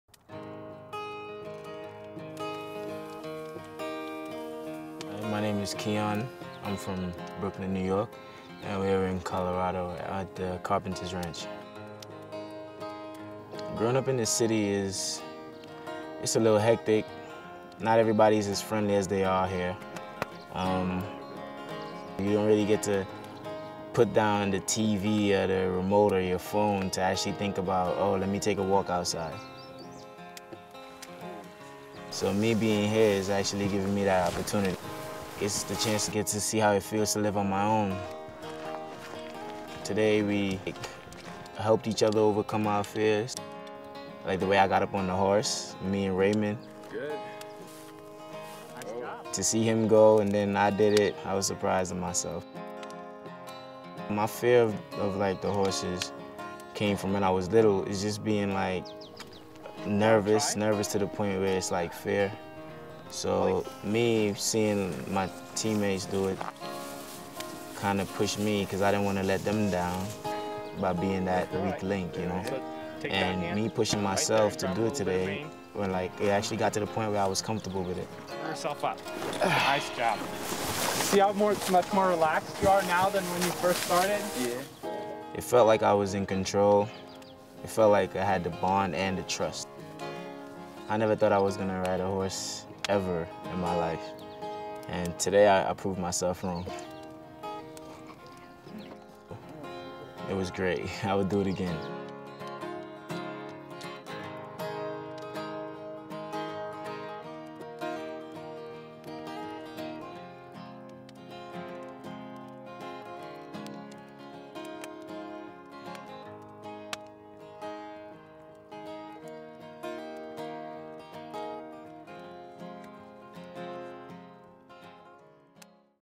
Life on Carpenter Ranch
Keon Flavius talks about his summer as a LEAF Intern with The Nature Conservancy on Carpenter Ranch in Colorado and his first experience riding a horse.